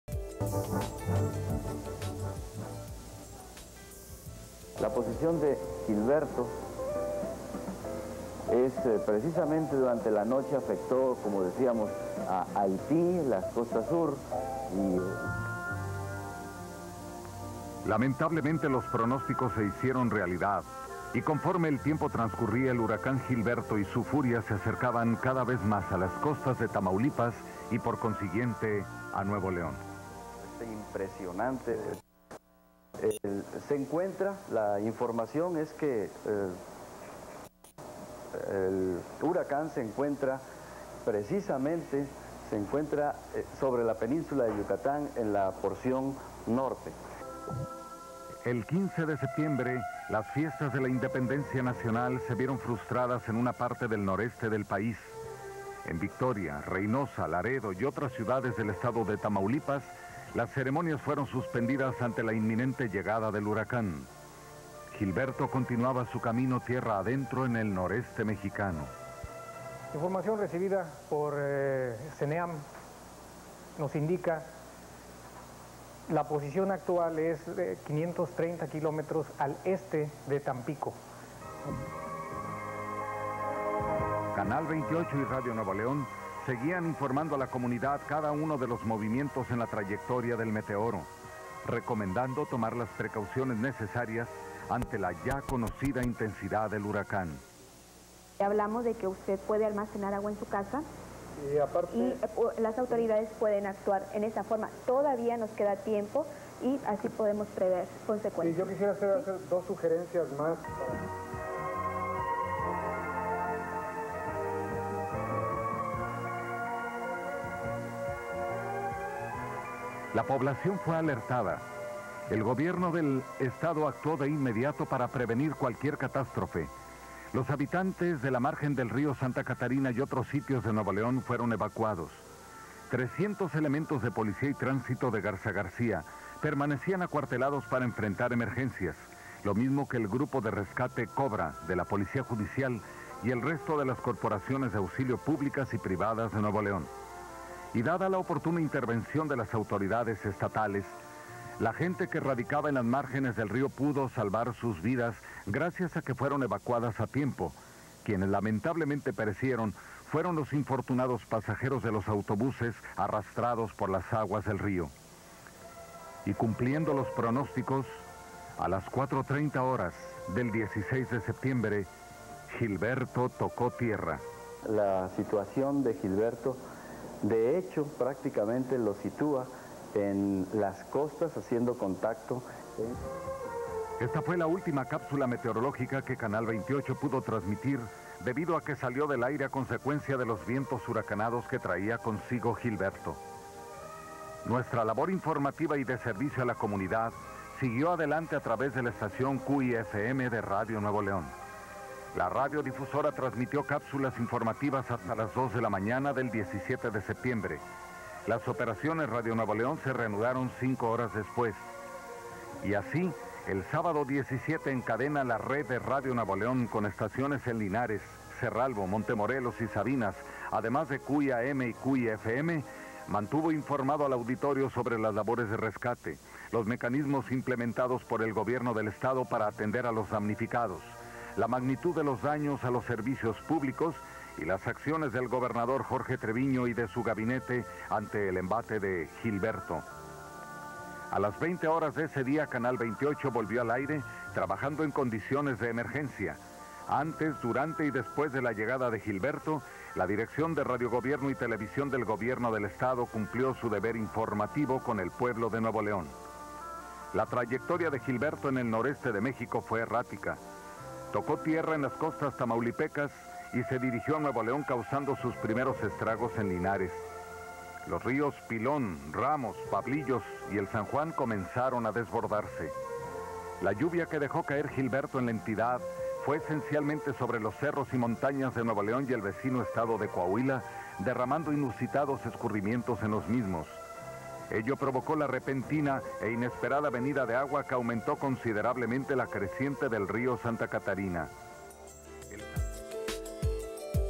Huracán Gilberto en Nuevo León 1988, recuento un mes después / 30 Aniversario
Este es un recuento realizado por Canal 28 recién un mes después de la entrada del huracán Gilberto a Nuevo León, con la cobertura que se hizo del embate del fenómeno tras tocar tierra en Tamaulipas la tarde del 16 de septiembre de 1988.

Gilberto, que azotó Monterrey y el estado hace justo 30 años, dejó una enorme tragedia, un número no determinado de muertos, miles de damnificados y pérdidas económicas cuantiosas, además del recuerdo de la crecida del Río Santa Catarina, que se llevó todo a su paso.